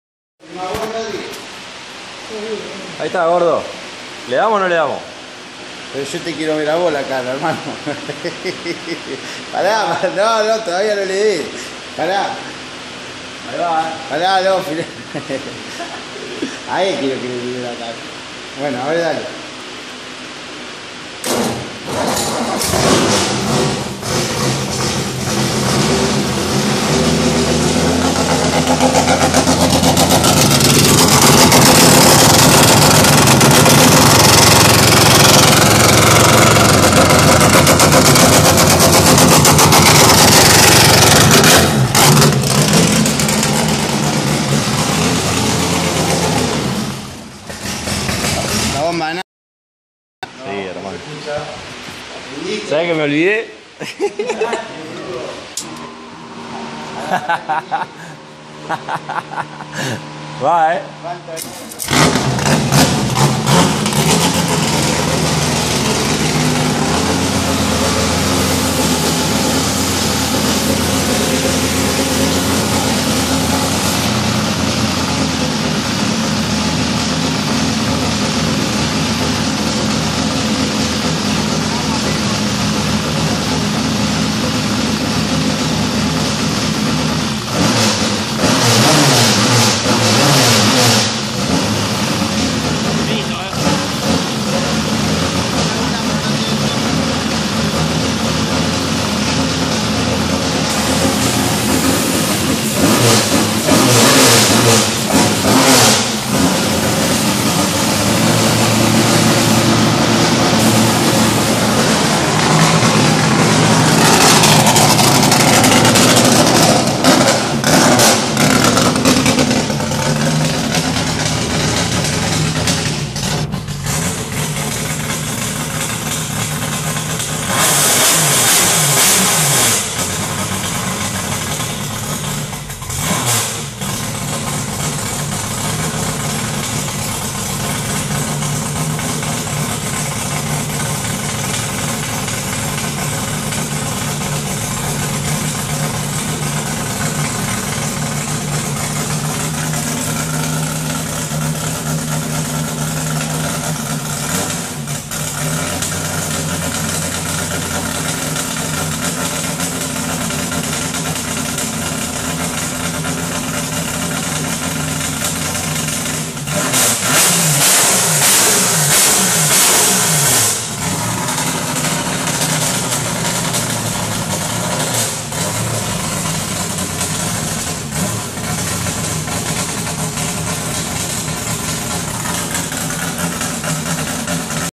141129 1ra puesta en marcha by piedigrossi
1ra puesta en marcha del motor by Piedigrossi para el Hulk.